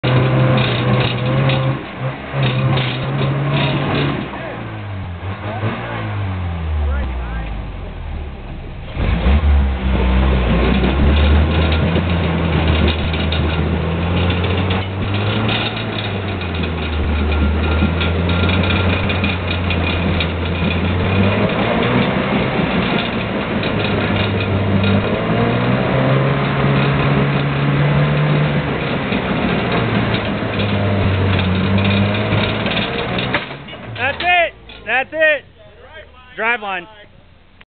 Broken yota U Joint at Table Mesa
Jovan brakes his u joint flange.  Too much axle wrap and the spring clip comes apart allowing pinion angle to change causing distruction..